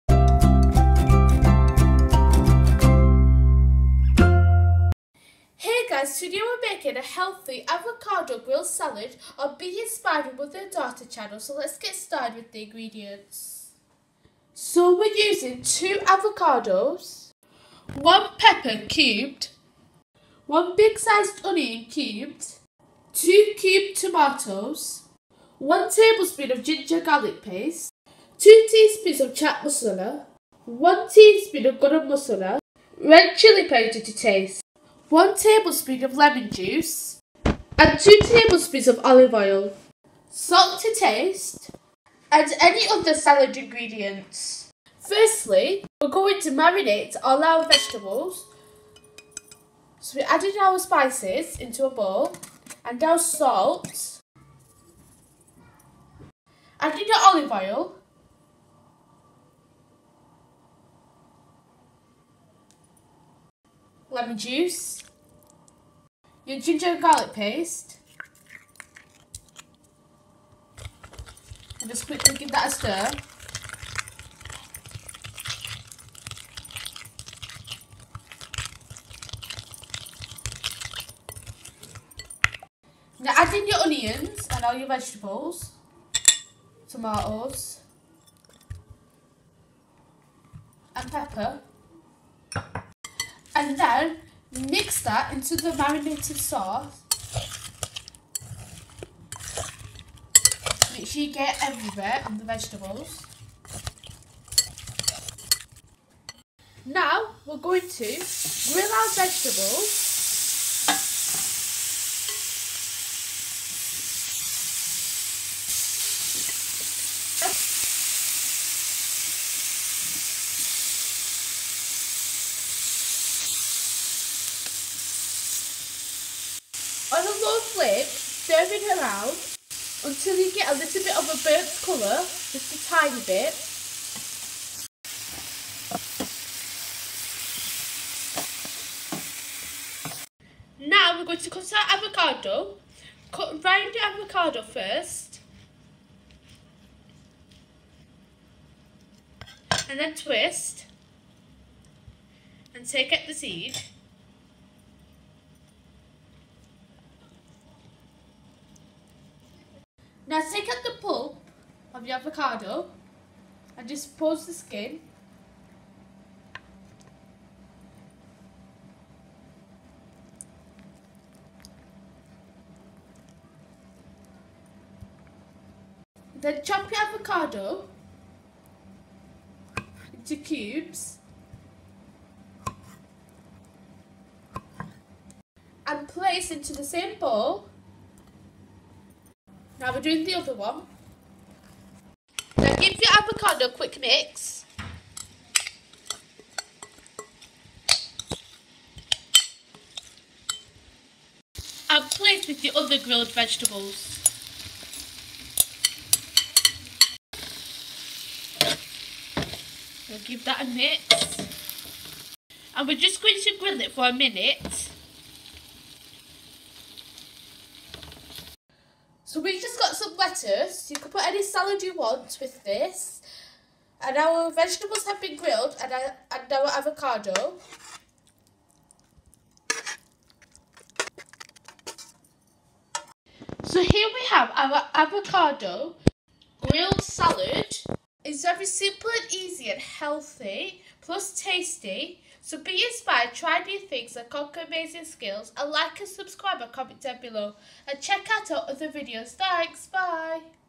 How to make healthy avocado grilled salad
This is a delicious yet healthy savoury salad :How to make healthy avocado grilled salad. So be inspired, try new things and conquer amazing skills.